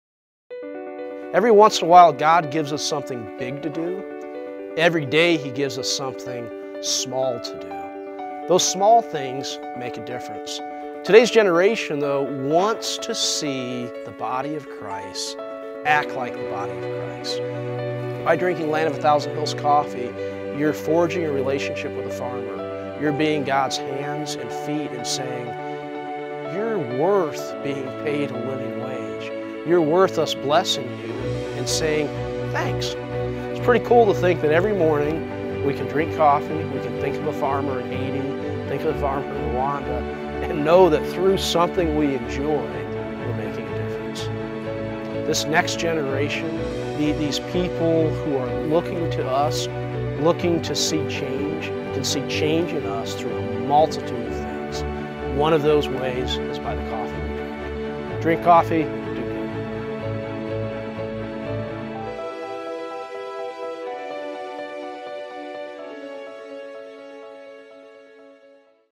Small Things, Big Impact
Hear Land Of A Thousand Hills’ Founder, Jonathan Golden’s views on how even the smallest tasks can have a life-changing impact on the people around you.